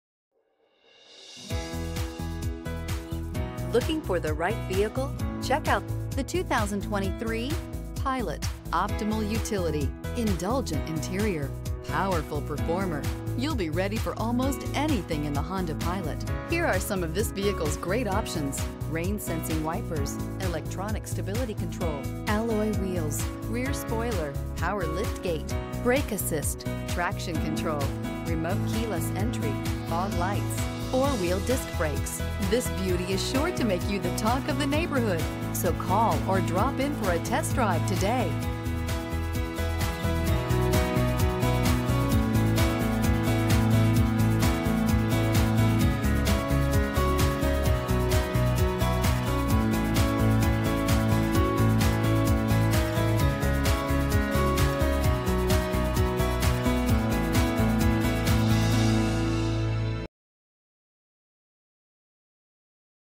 2023 Honda Pilot Schlossmann Honda City Inventory 31612
2023 Honda Pilot Elite

3450 South 108th St.

Crystal Black Pearl 2023 Honda Pilot Elite AWD 10-Speed Automatic 3.5L V6 DOHC 24VbrbrRecent Arrival!